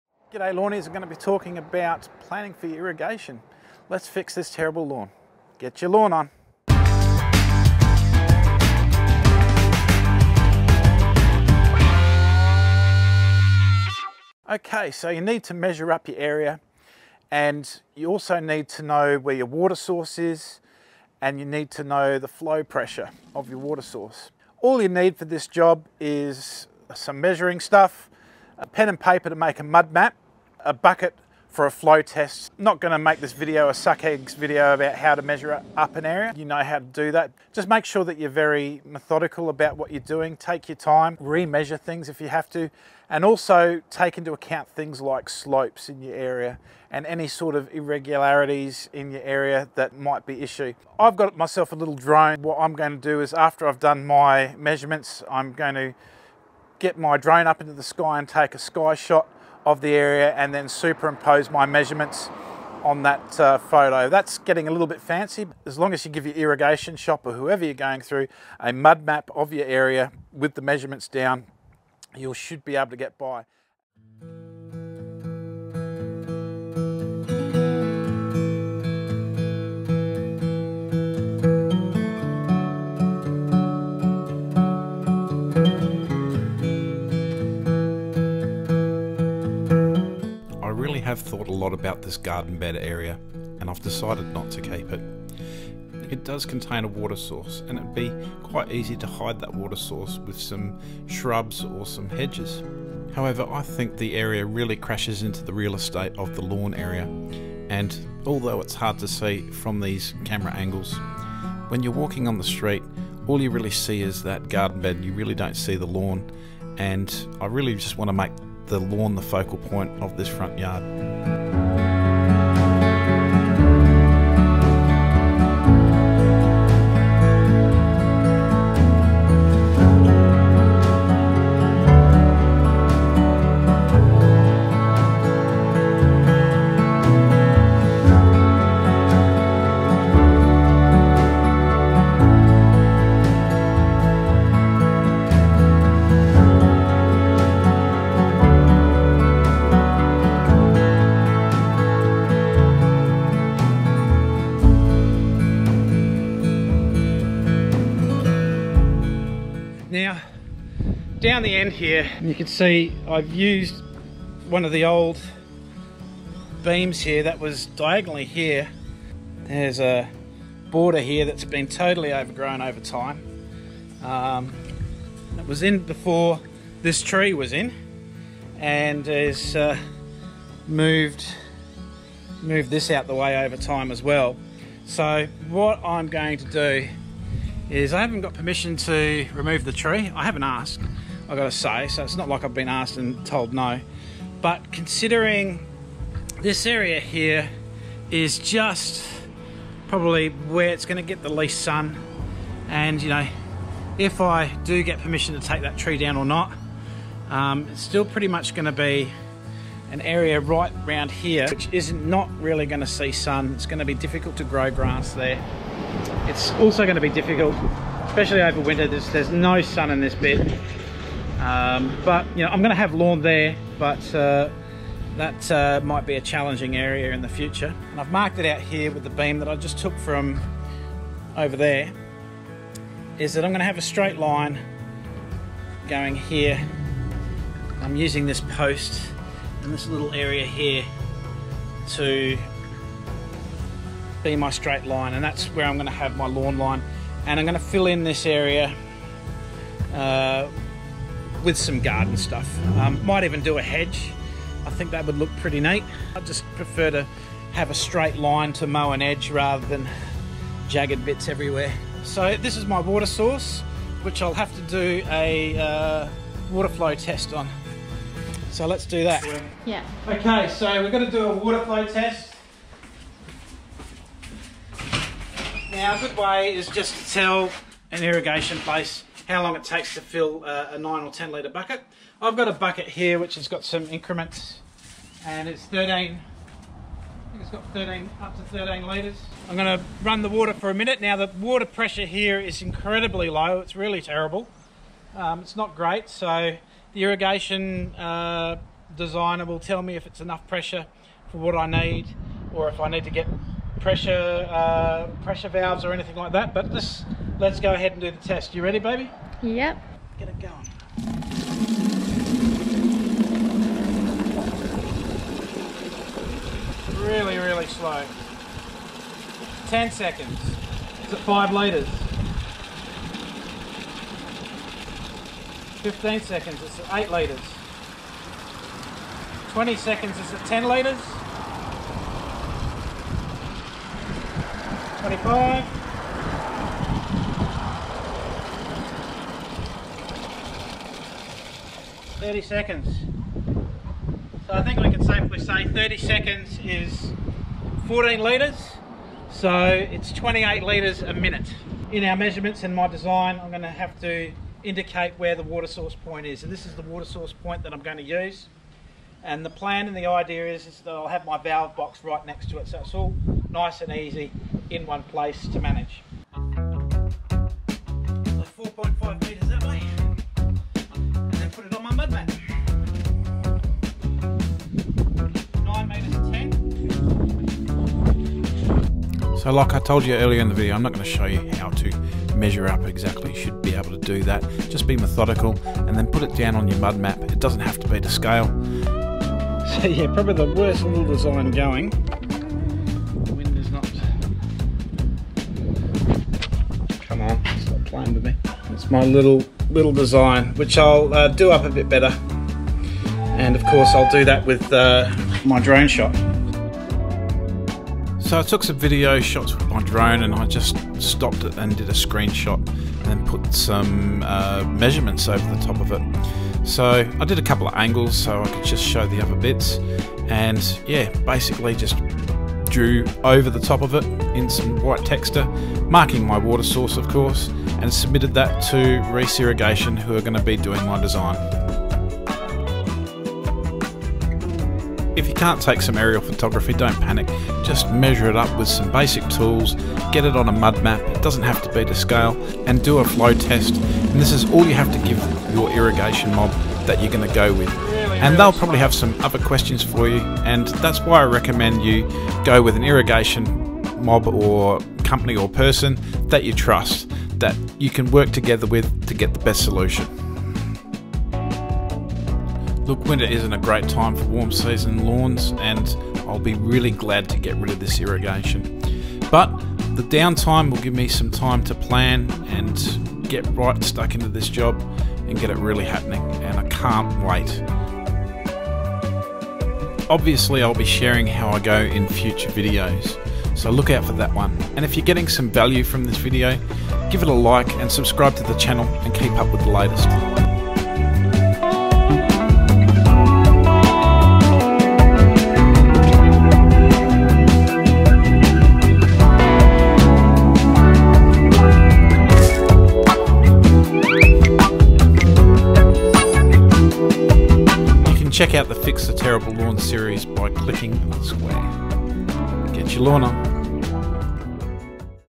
Irrigation Plan // Fix a Terrible Lawn Part 9
Water is vital to lawn and if you are serious about saving water, money, time and having a great lawn by watering properly and efficiently, irrigation should be on your radar.

So it is winter in Port Augusta and while we sometimes get days in the low 20's, the overnight/early mornings can be as low as -2 (thus far). The lawn (now mostly Santa Ana/Bermuda couch) certainly is not doing much. Hardly need to mow it. I have been doing some weekly Launch+/Root Builder+ treatments followed by Charger/Root Builder+ the following week. All at a low level but there isn't much happening to be honest. Certainly it looks healthier but the filling in isn't going to happen till spring growth gets started.

That is fine, the time off lets you plan the next season and irrigation is my plan. While I didn't go into it much this lawn has some irrigation but it is a poor fit for the space. The sprinklers are pop ups but they sit above the ground and I can't even mow without having to remove them every time I mow.. I am totally over that so I am making a new design.

So all you really need is a tape measure, some paper and a pen, and a bucket that has some markings or that at least you know how many litres it holds to fill it... oh and you'll need a watch to time it too I guess.

Measure your area as best as you can, mark the water source within your plan and do a flow test with your water source tap... And then take it to an irrigation shop or person that you can really sit down and discuss your options with.

I'll give a shout out to Reece.com.au in Currie Street ADELAIDE. They are my go to. I have dealt with other irrigation mobs before that just chase the cheapest prices and option to get the sale and forget about the customer with hope they'll just work it out. So choose your people carefully.

I have done a few irrigation installations now and while I am no expert, I have learnt some things along the way. If you decide to have a go at installing yourself, it is difficult work (earth moving always is) but the rewards are awesome and there is nothing like DIY satisfaction. If you need it to be installed for you, discuss with your irrigation supplier as to who they recommend, they'll have a great network for sure to help you out.

I really liked the Weathermatic gear with my last design. The pop ups are great and I'll put Hunter MP Rotators sprinkler heads on because they work, are easy to adjust and seem to be pretty much maintenance free after I have set them.

For a controller, I will probably just go a Proline. The Smartline, Smart Controller that connects to the mobile net and rain gauge are all really smart features but it hardly rains at Port Augusta and I have to admit I hardly water my lawn after a renovation so I don't really use the phone app and mobile network all that often to justify the expense. Remember this house is a rental so I don't want to go overboard.

Well enjoy this video, Irrigation Plan // Fix Terrible Lawn Part 9. Next week I'll submit my design to Reece Irrigation in Adelaide and remove the garden bed for the next video.

I am planning to do the trenching and install on the 18th and 19th of July, so the trenching will look crappy for a few weeks but then spring will be upon us and I'll be able to renovate finally, level up everything and look towards great improvement with the growth.

Get your lawn on!!